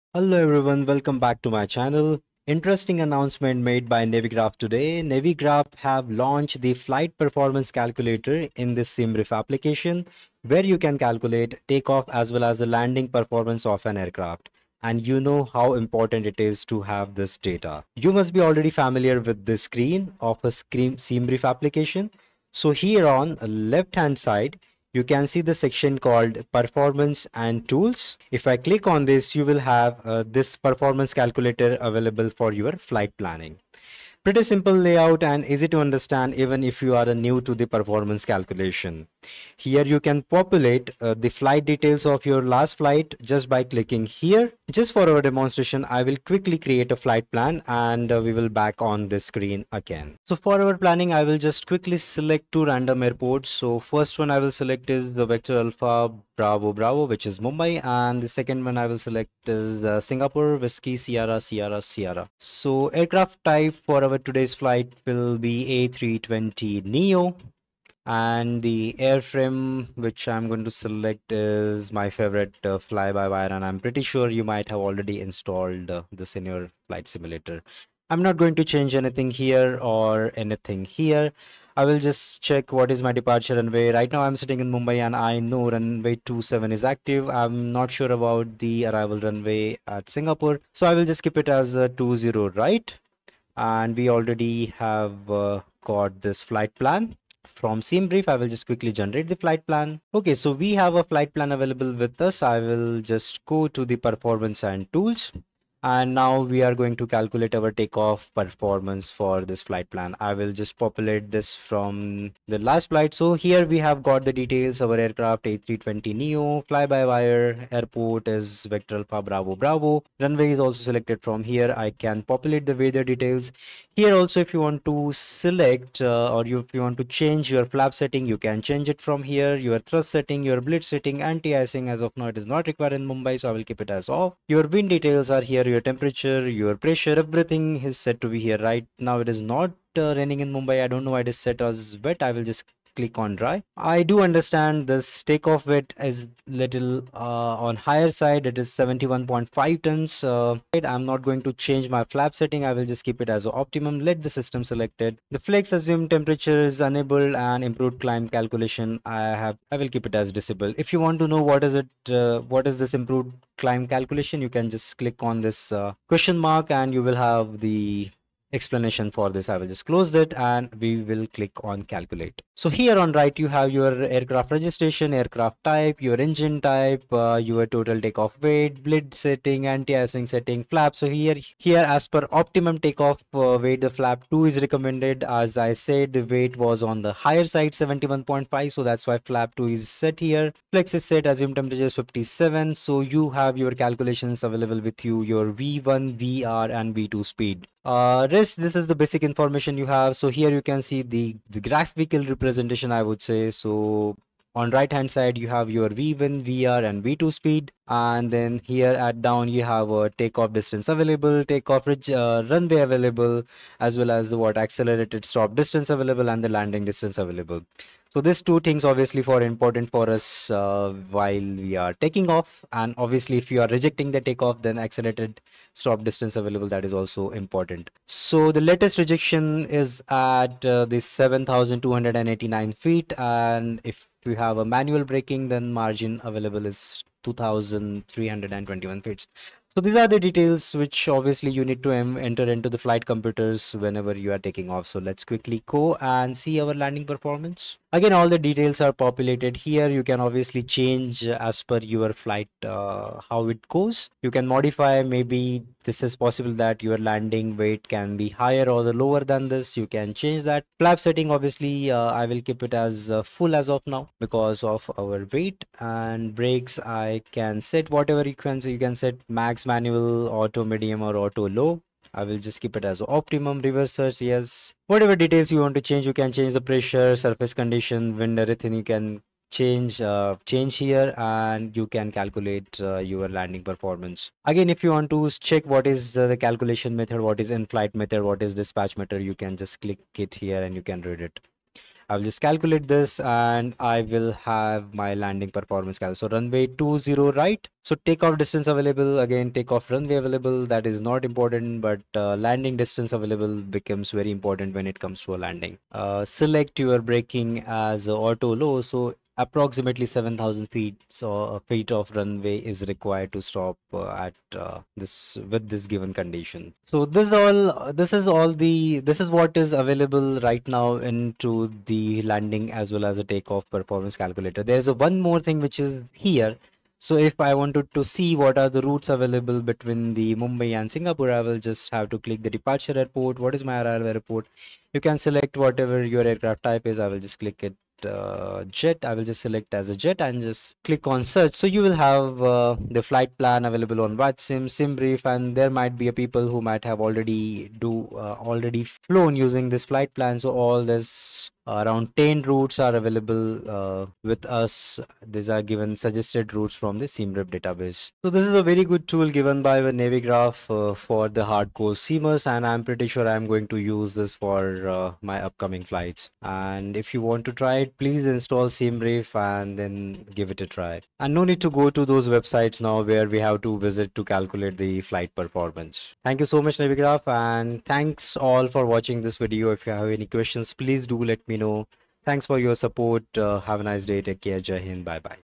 New Navigraph / SimBrief Performance Calculator | Takeoff and Landing Performance of Aircraft
Video is about new feature launched by Navigraph in simbrief app for flight performance. Calculate takeoff and landing performance for your fight to add more realism in simulation.

@Navigraphteam Thank You 😊